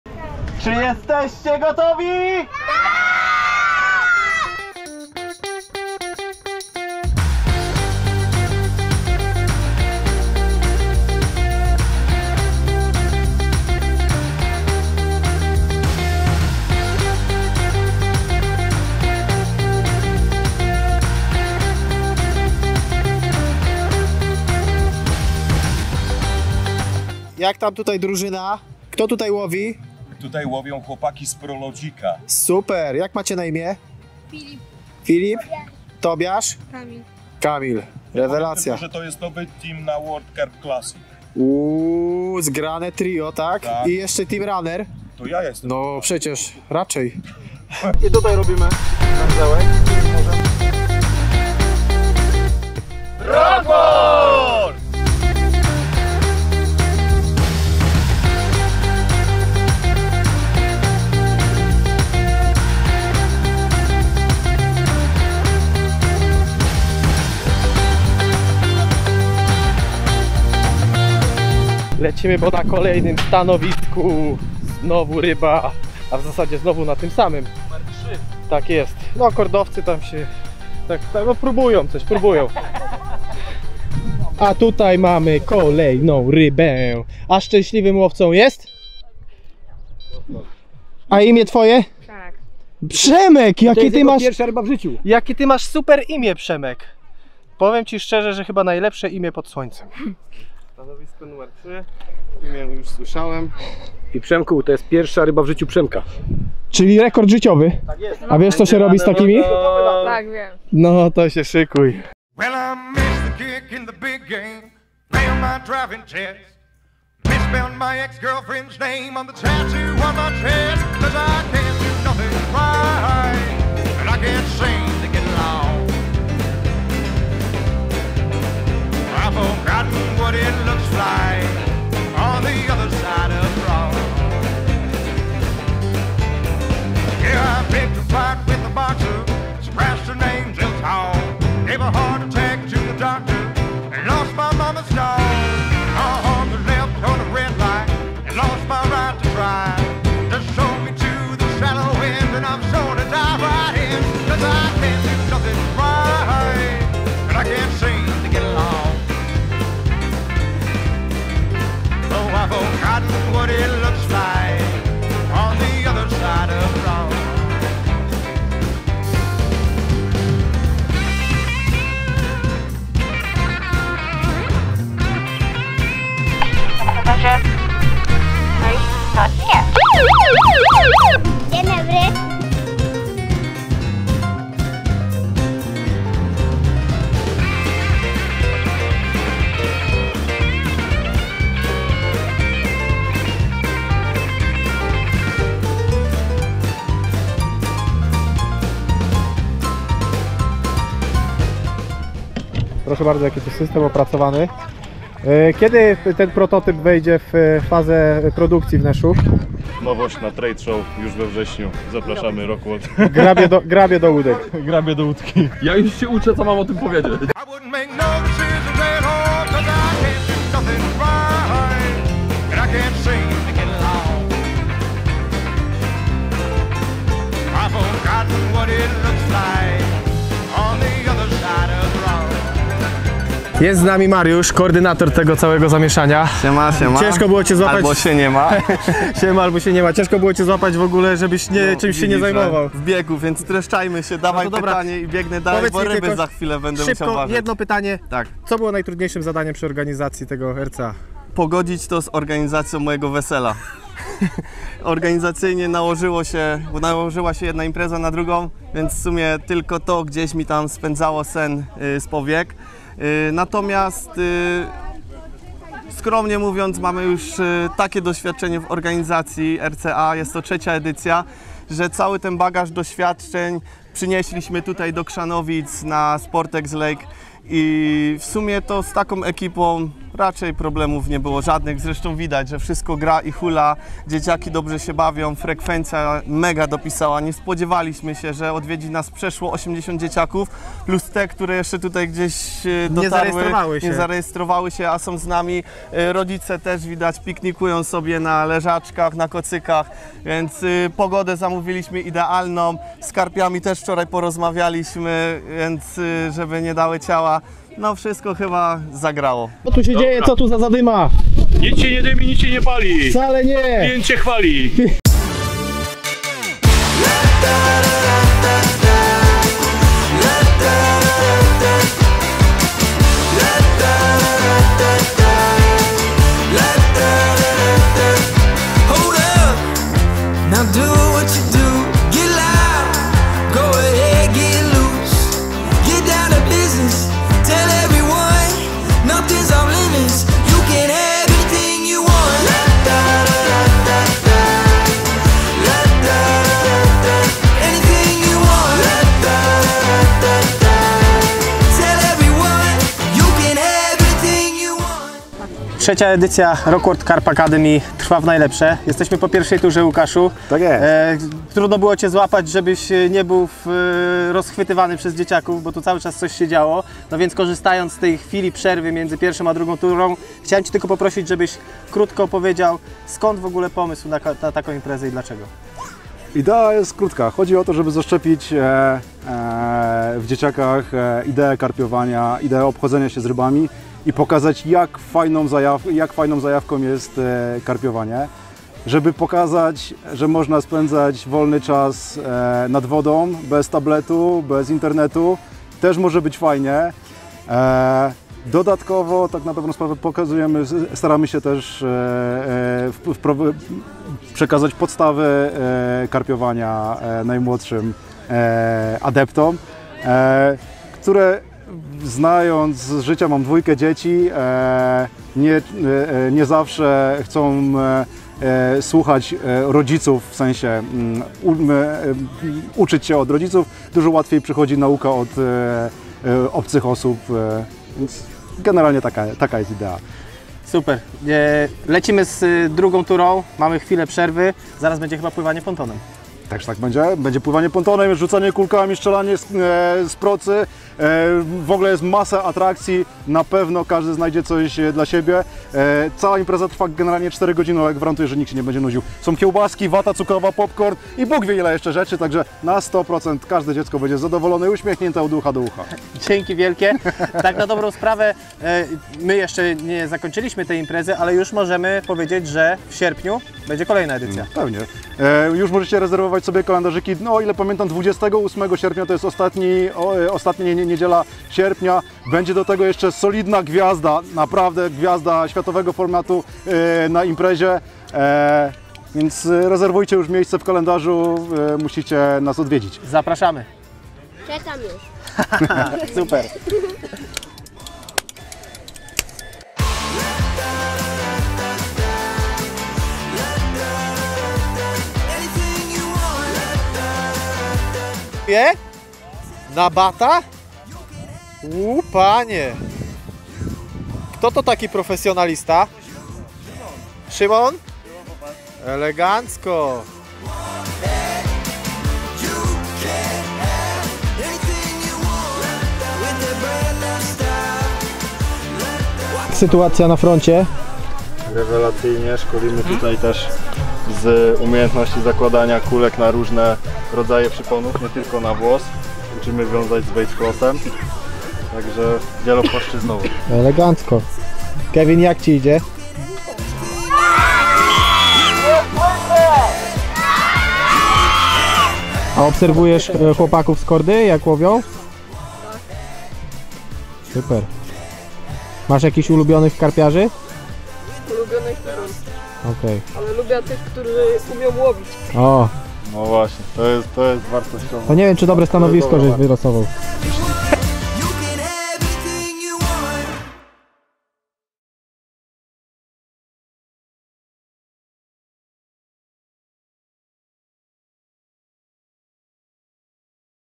Rockworld Carp Academy Junior - relacja z imprezy
Co to była za impreza?!! Kto nie był, niech żałuje!

Mamy dla was krótki materiał filmowy podsumowujący V edycję RCA, zapraszamy do oglądania i już dziś informujemy, że niebawem kolejna edycja RCA Junior! Kiedy? Ostatnia niedziela sierpnia, w sam raz na zakończenie wakacji. Więcej informacji już wkrótce, obserwujcie nas!